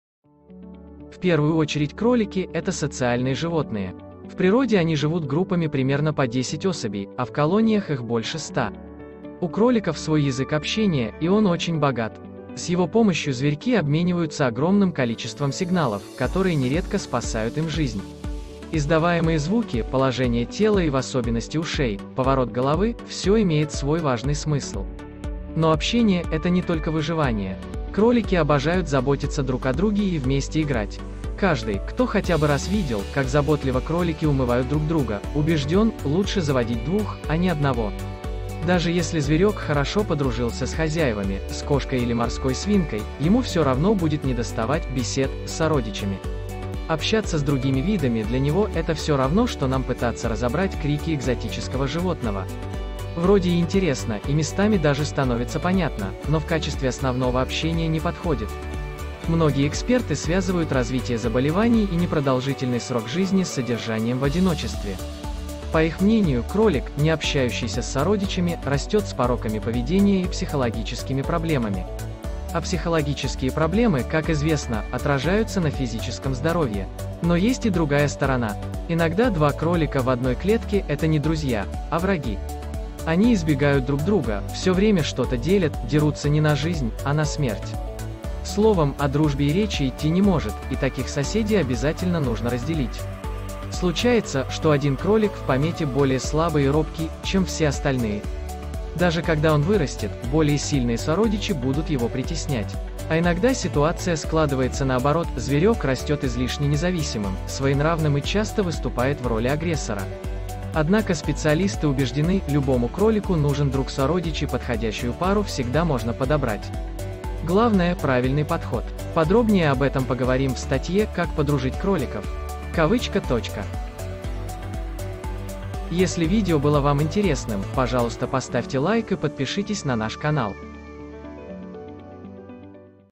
Два кролика в одной клетке: за и против
Лучше завести одного кролика или сразу нескольких? Насколько это социальные грызуны? Как они уживаются с сородичами? Рассказываем об этом на портале Хвост Ньюс.